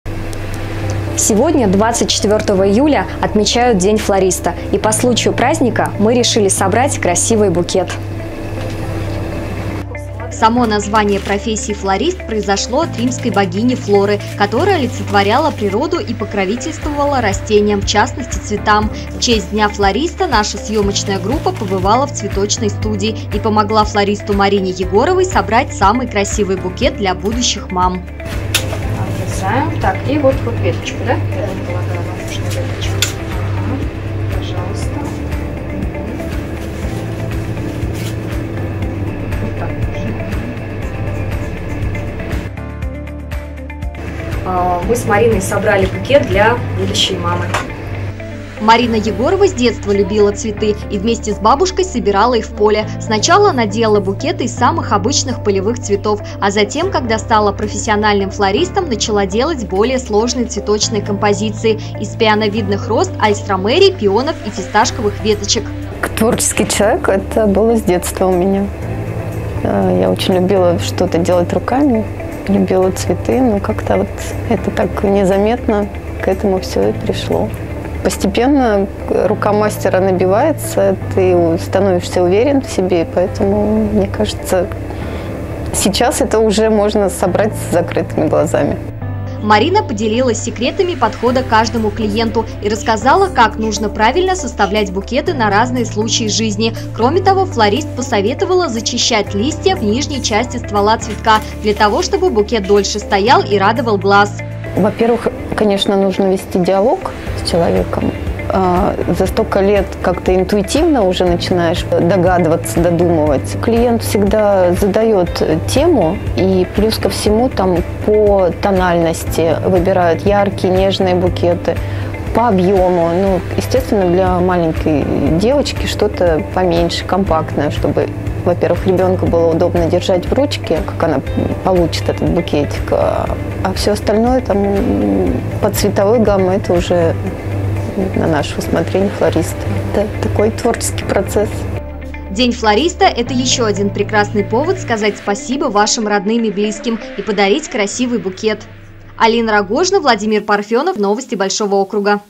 Сегодня в России отмечают День флориста. Учимся собирать красивый букет из цветов
Как составить яркий букет своими руками из разных цветов? Наша съёмочная группа побывала в цветочной студии и собрала вместе с флористом Мариной Егоровой красивый букет для будущих мам! Как это было, расскажет Алина Рогожина.